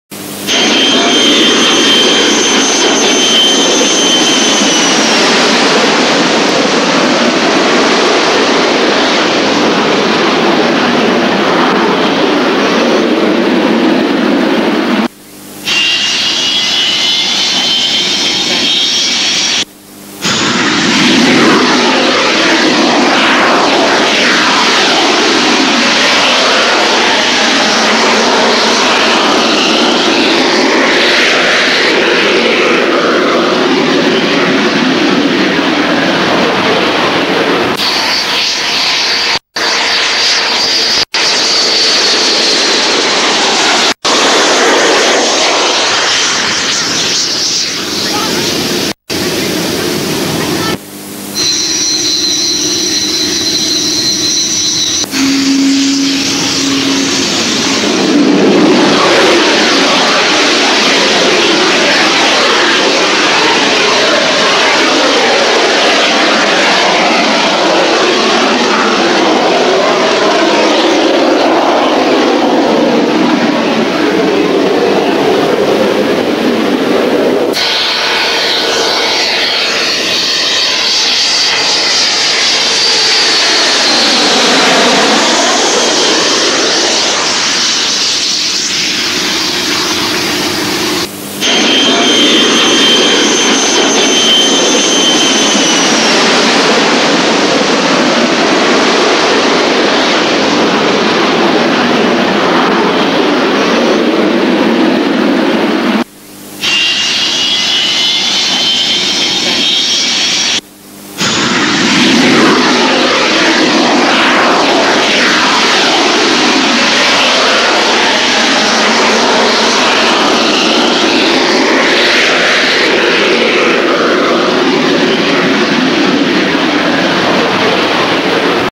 Jets at Valley Airforce Base - 21st August 1984 - Colour Sound Super 8
Jets at Valley Airforce Base - 21st August 1984 - Colour Sound Super 8 Cine film.

Includes excellent shot of a Buccaneer aborting its landing.